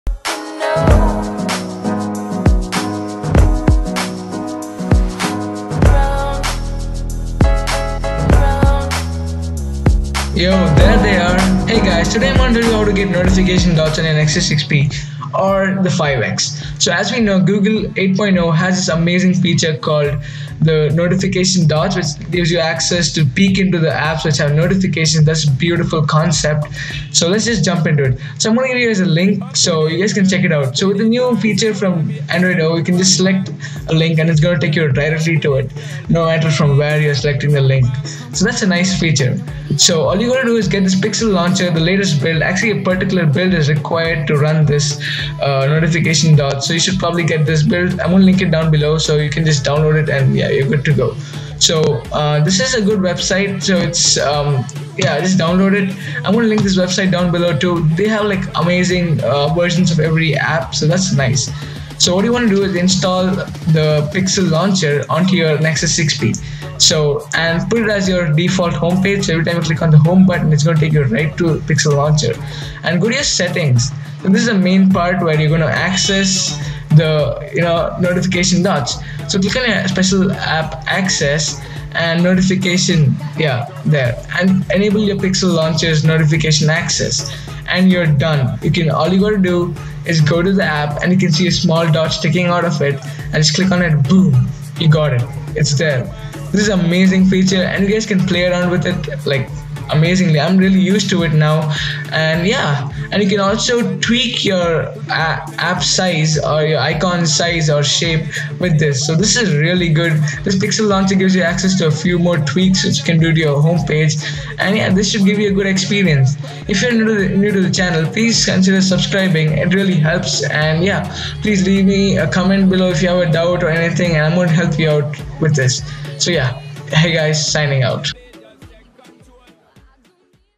HOW TO GET ANDROID OREO NOTIFICATION DOTS FOR THE NEXUS 6P!
HEY GUYZ!! I GOT A LOT OF QUESTIONS REGARDING THE NOTIFICATION DOTS, AND I MADE A REALLY EAY TUTORIAL FOR YOU GUYZ TO FILLOW TO UNLOCK THE NOTIFICATION DOTS ON YOUR ANDROID O SUPPORTED DEVICES! I HAVE INCLUDED A FEW LINKS AND I WILL BE LONKING THEM DOWN BELLOW HOW TO GET ANDROID OREO NOTIFICATION DOTS FOR THE NEXUS 6P!